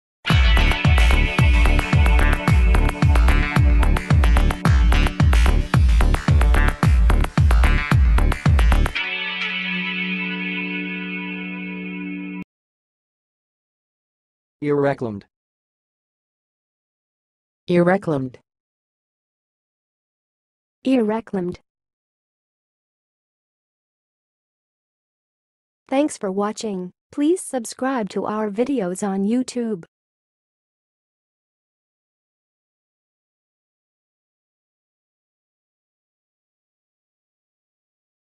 How to say "irreclaimed"! (High Quality Voices)
Watch in this video how to say and pronounce "irreclaimed"!